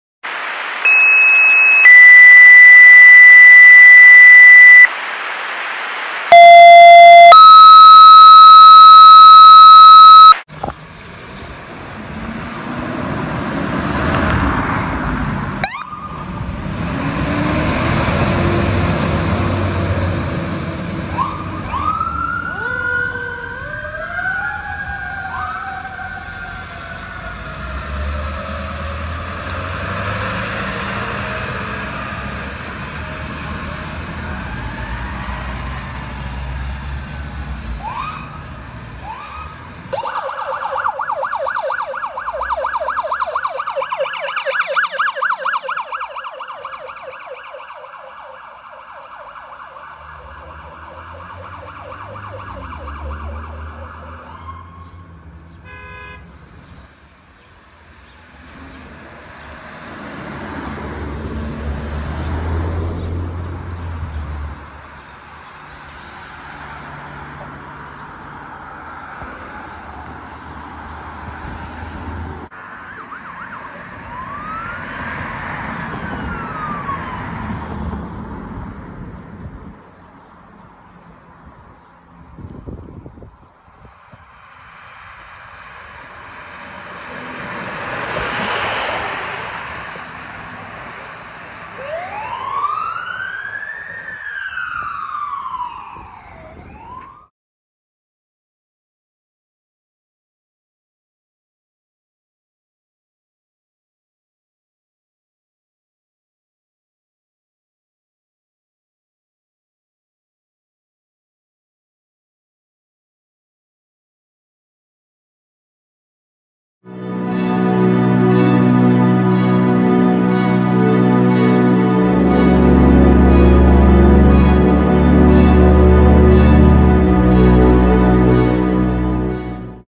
Mount Kisco Fire Department Engine 106, Utility 13, Rescue 31 and MKVAC 70B2 Responding
Here you will see Mount Kisco Fire Department engine 106, utility 13, rescue 31, and Mount Kisco Volunteer Ambulance Corps 70-B-2 responding to a vehicle into a pole.

All footage seen here was taken by me, on 10/12/24.

Engine 106: 2021 Seagrave Custom Rescue Pumper

Utility 13: 1998 Ford/PL Custom

Rescue 31: 2005 Ford F550/EVI

70B2: 2012 Ford E450/PL Custom